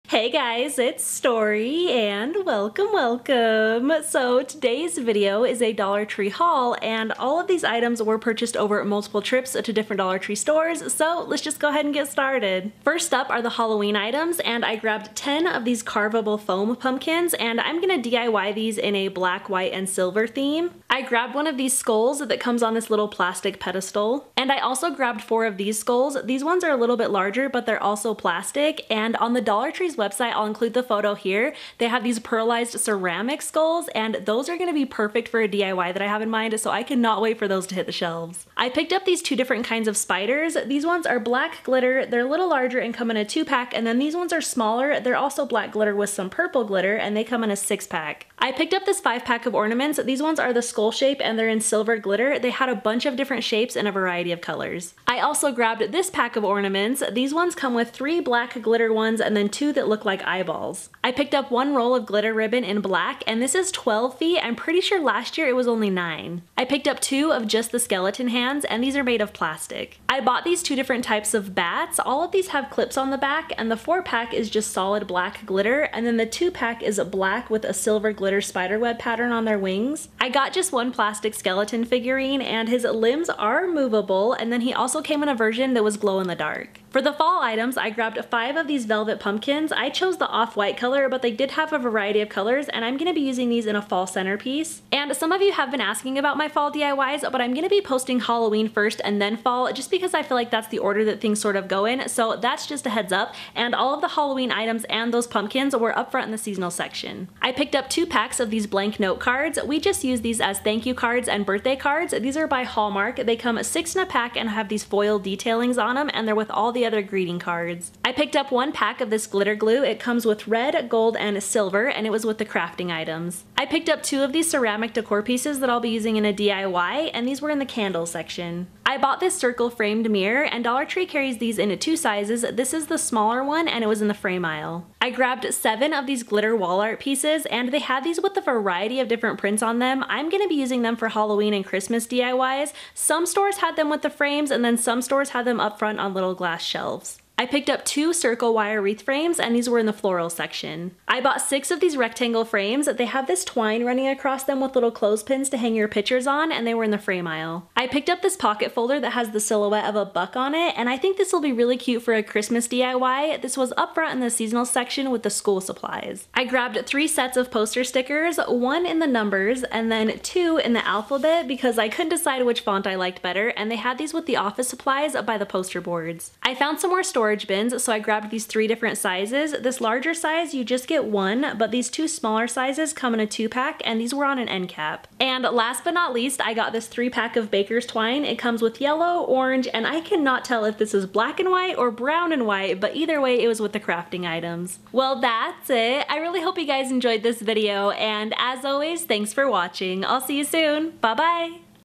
NEW DOLLAR TREE HAUL HALLOWEEN 2019 FINDS
It's another Dollar Tree Haul! The first wave of Halloween finds have hit my Dollar Tree stores. I got some supplies for my Halloween diys which I’ll be posting first before my fall projects. I really hope you enjoy!

I’m Storee and let’s be friends! PS I'm mostly on Instagram ;)

FTC- I am not being paid by any brands/companies mentioned or shown in this video.